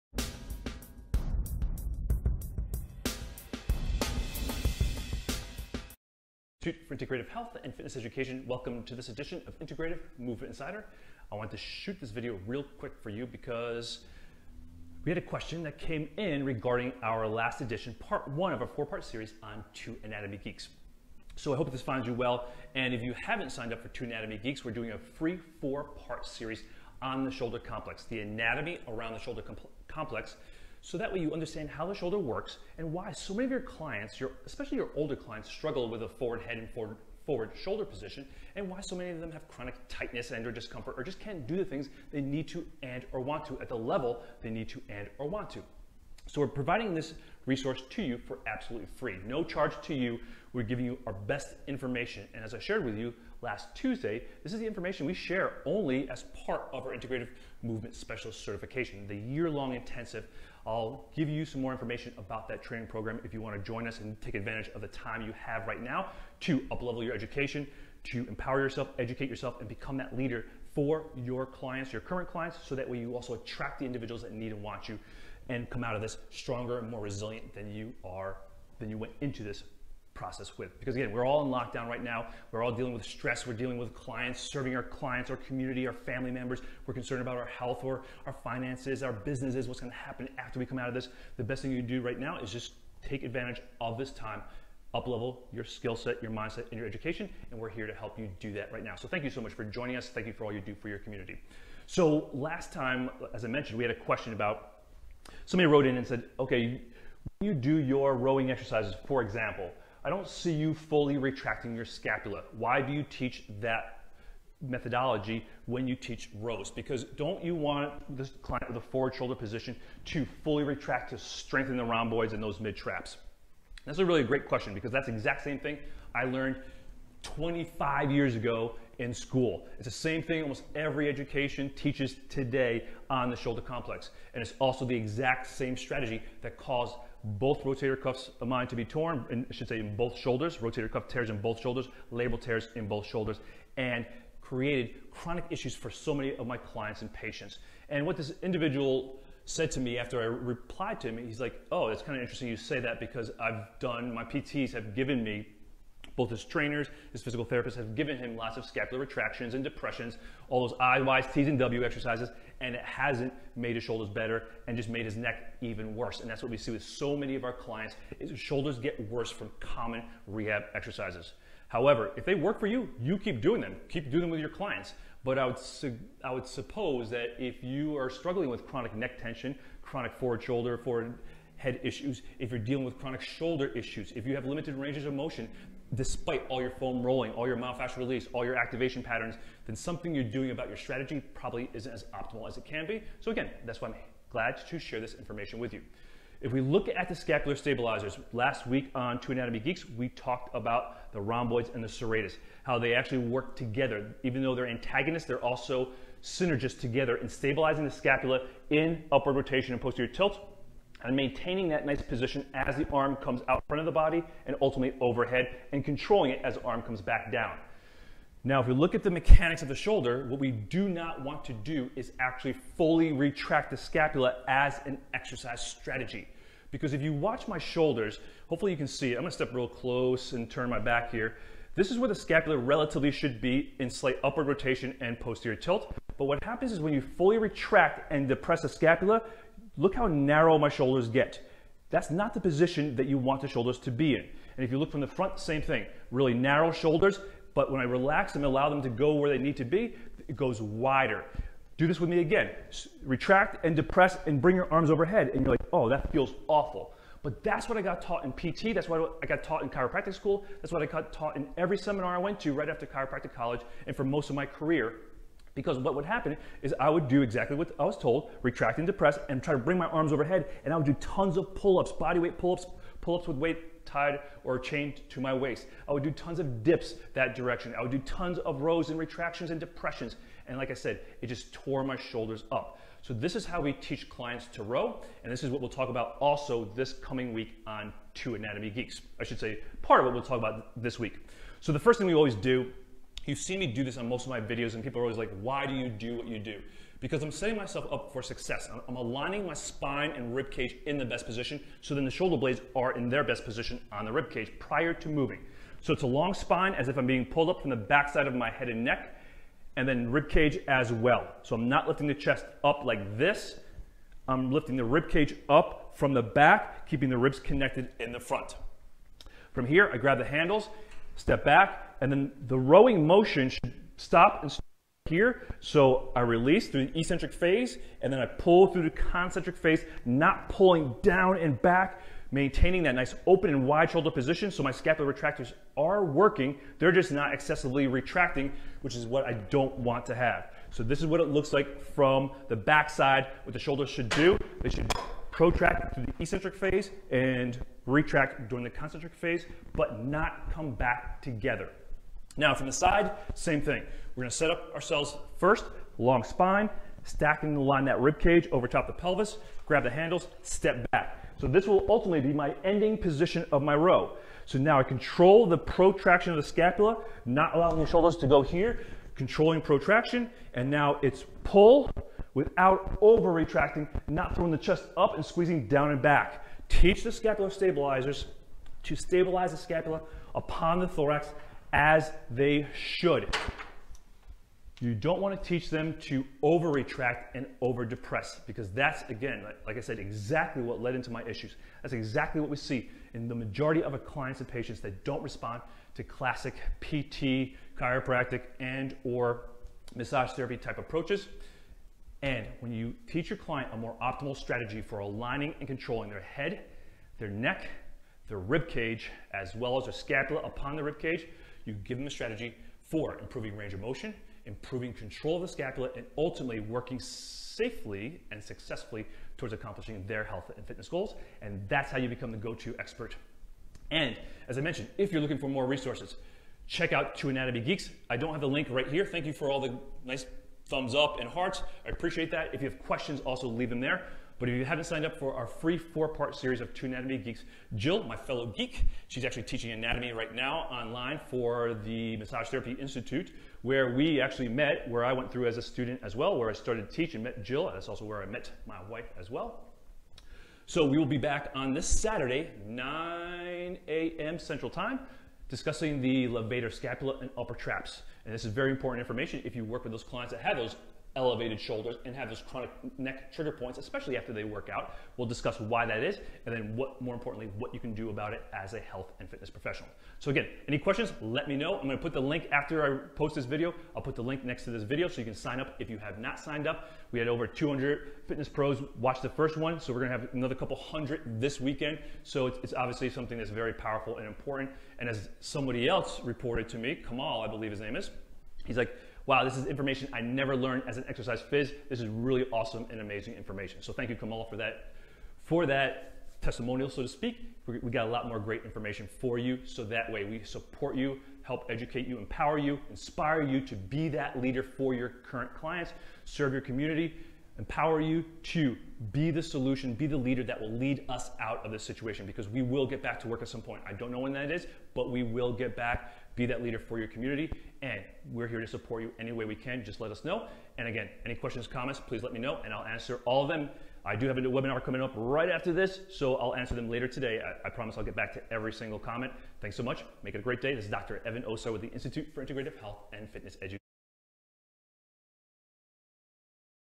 The Shoulder Complex: To Retract or Not to Retract
Retract or not to retract. That is the question Dr. Evan Osar - Corrective Exercise Solutions to Common Hip and Shoulder Dysfunctions - is addressing this week on Two Anatomy Geeks anatomygeeks.

🆓Part 2 of the 4-part series on the Shoulder Complex: Levator Scapula and Upper Trapezius. Implications in the Forward Shoulder and Forward Head position. Shoulder Anatomy.